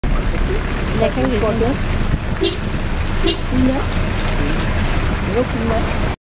緬甸 - 蒲甘、仰光廟來廟往之旅 ~ Apr 2014 ~ 在蒲甘寺廟區遇上牛群之後又遇上羊群
歡迎我的旅遊網誌看"緬甸 - 蒲甘、仰光廟來廟往之旅"的遊記